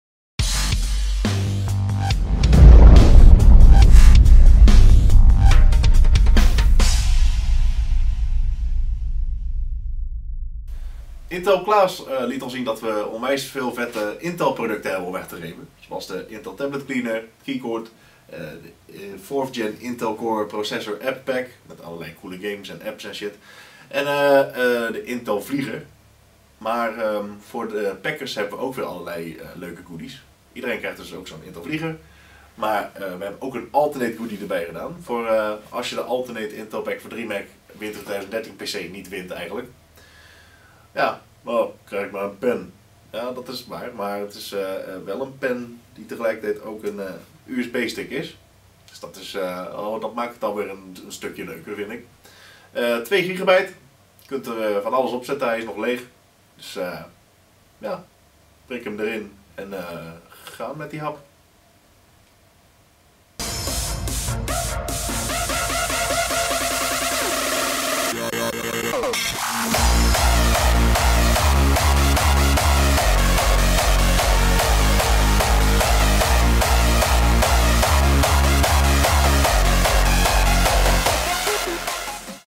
Jeroen Presents: Alternate USB Goodie
We just got our first goodie in and it's a pen. A pen you might think. But this is no ordinary pen...